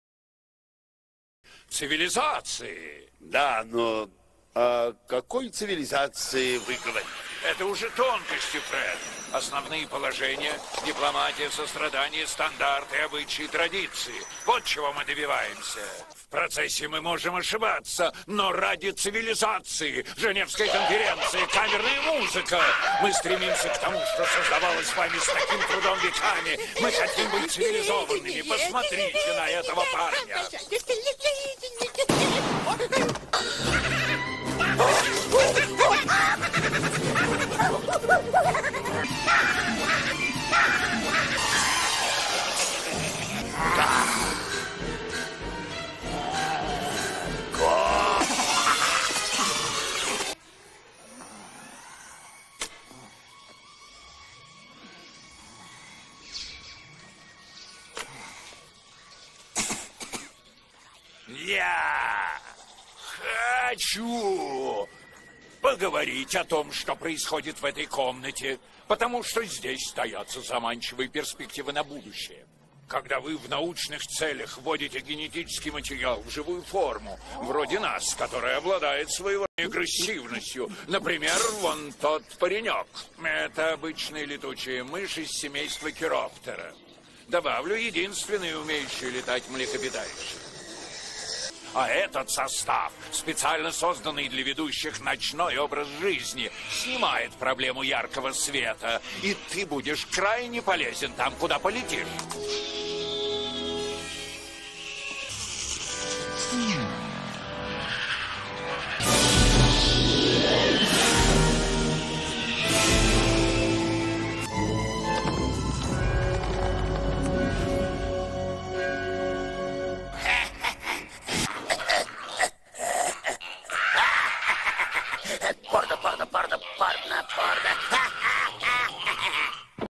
Вакцина будущего не всем полезна, но некоторые улетают после нее.
Вакцина, гремлены, изменение на биологическом уровне.
Как улетел гремлен и превратился в горгулью от вакцины.